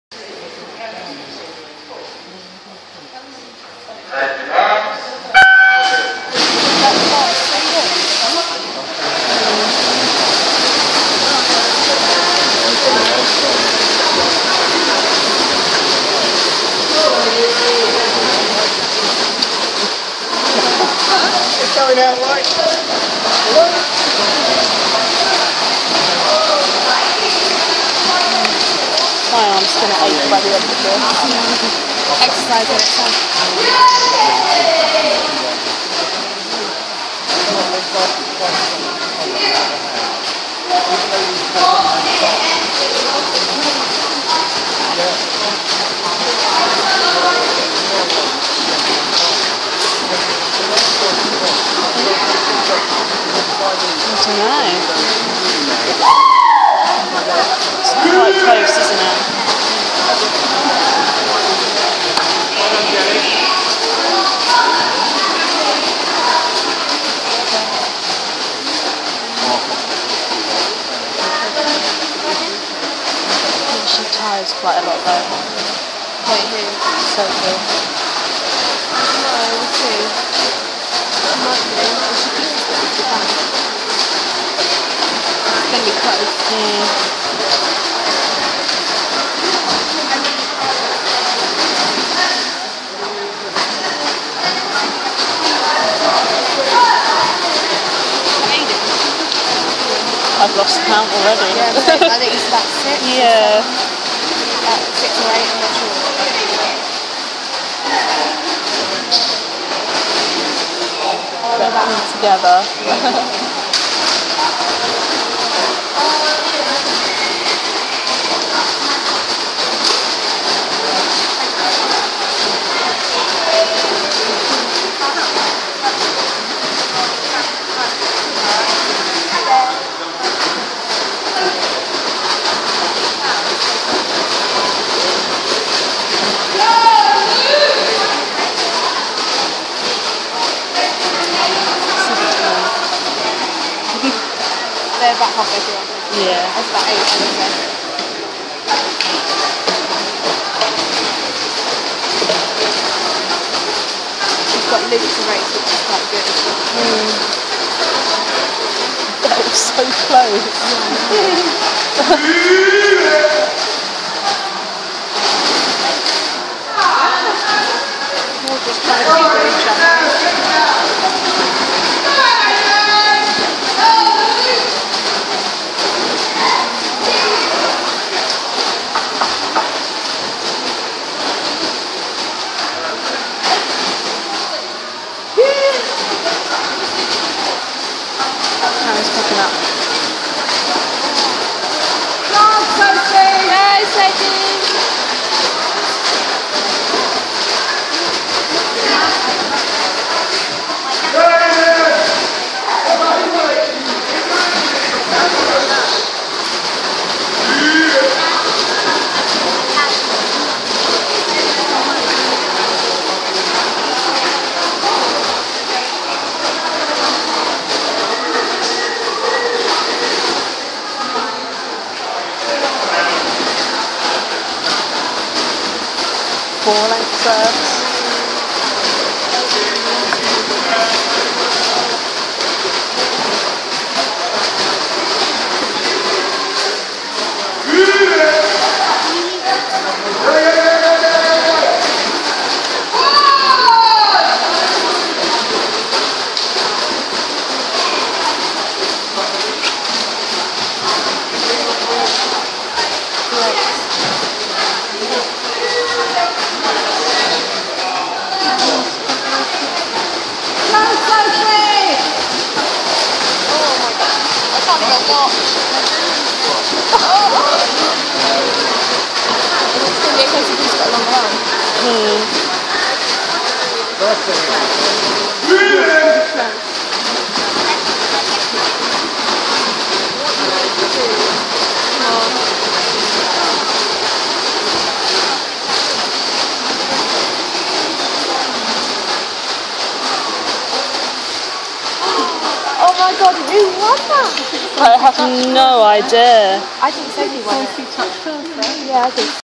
Shiverers Club Champs 2013 - 400 Free
Shiverers Club Champs - 24/07/13
Lane 1 -Luke Moore
Lane 2 - Soph Quach
Lane 3 - Aidan Cannon
Lane 4 - Karen Chan